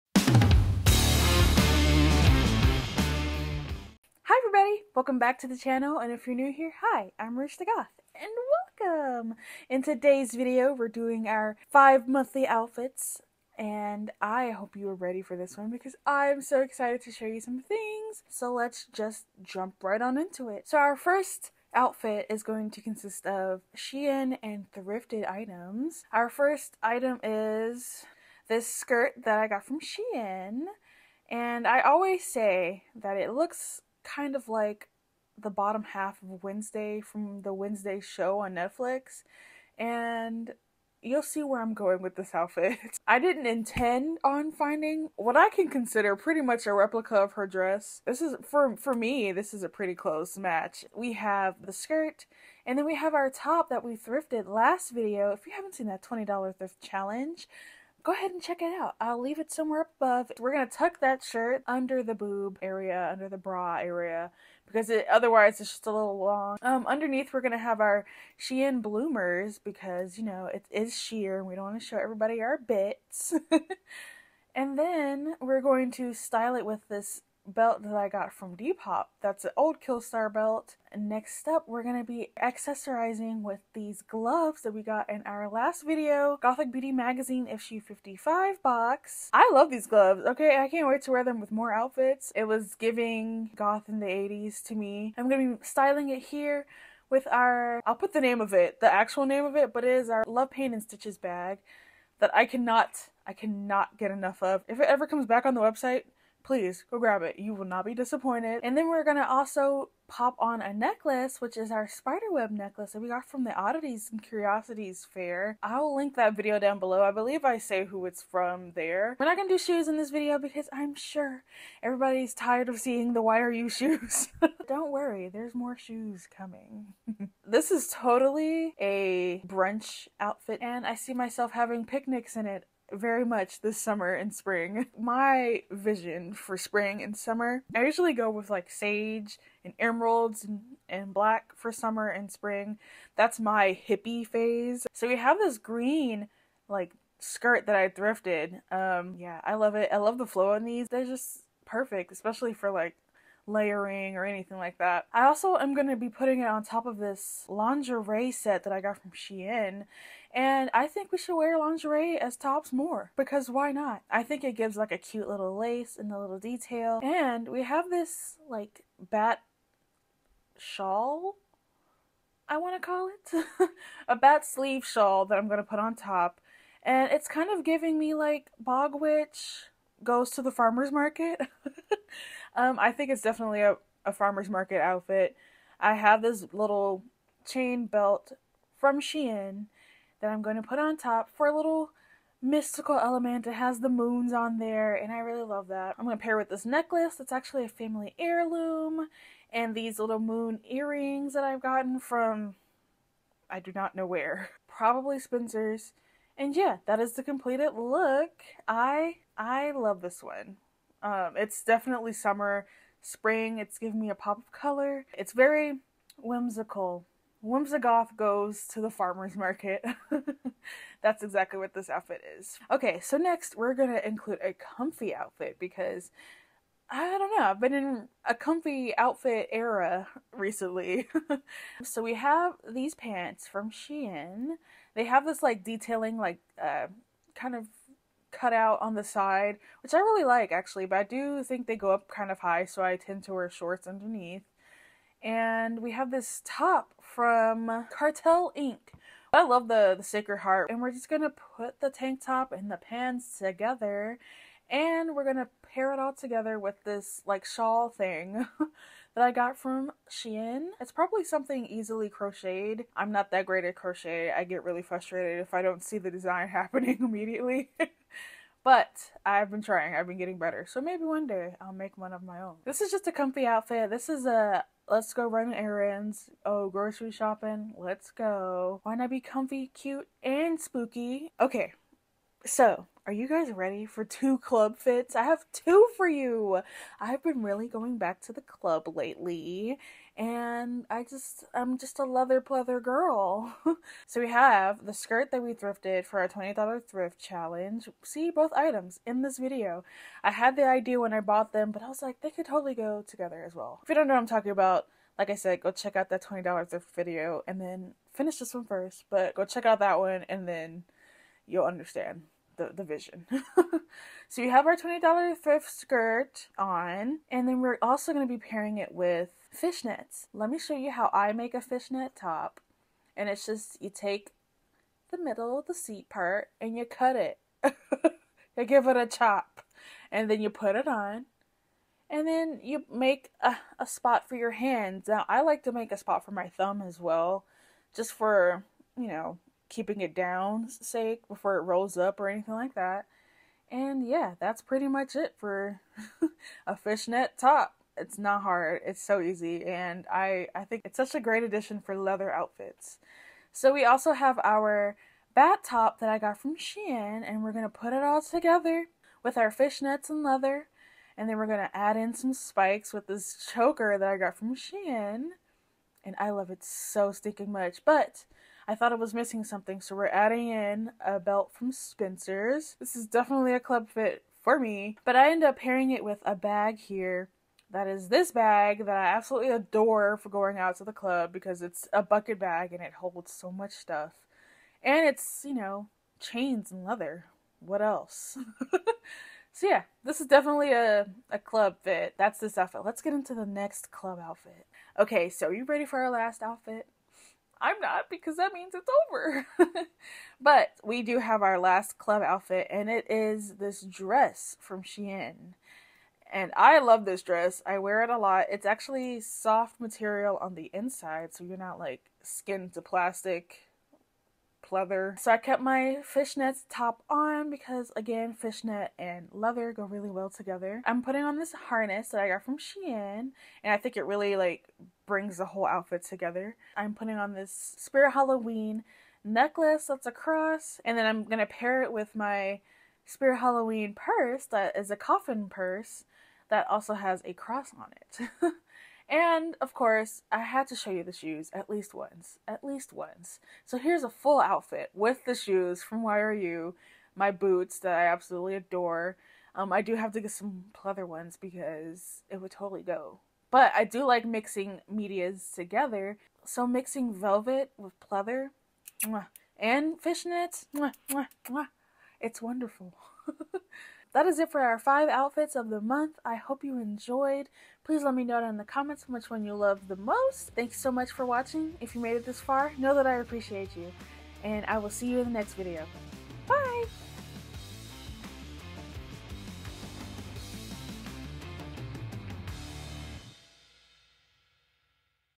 Monthly Plus Size Goth LookBook!!
Hey there lovelies, we are doing our March monthly lookbook! I'll be showing you 5 outfits I'm dying to wear as we start to transition to spring/summer!
~ Rouge
🕸 If things are sold out keep looking in the recommended there might be a similar or new gem to find!

My Look:
🖤LashyDoodle
🖤Beauty Store Lashes
🖤Old Colourpop lip color
🖤Old Colourpop liner

Look 1⚰️
🖤Skirt (similar)
SHEIN ICON Plus Size Mesh & Lace Patchwork Half Skirt
🖤 Bloomers
ROMWE Kawaii Plus Bow Front Lace Trim Ruffle Hem Shorts

Look 2⚰️

🖤Shawl (sold out)
ROMWE Fairycore Plus Size Black Hollow Out Cardigan Sweater

🖤Night Dress
Plus Contrast Lace Split Thigh Velvet Night Dress

🖤Belt
135CM Moon Decor Plus Size Corset Belt Retro Steampunk PU Belt for Coats and Dresses

Look 3⚰️
🖤 Shawl (similar)

🖤Pants
ROMWE PUNK Plus Cut Out Side Flare Leg Pants Without Belt (sold out)

Look 4⚰️

🖤Cami
ROMWE Goth Plus Lace Up Front PU Cami Top (sold out)

🖤Choker
ROMWE Goth 1pc Funky Ring & Spiked Decor Choker For Women For Daily Decoration

Look 5 ⚰️
🖤Dress
Zip Up PU Leather Cami Dress Without Gloves (sold out)

🖤YRU
CREEP - BLACK VELVET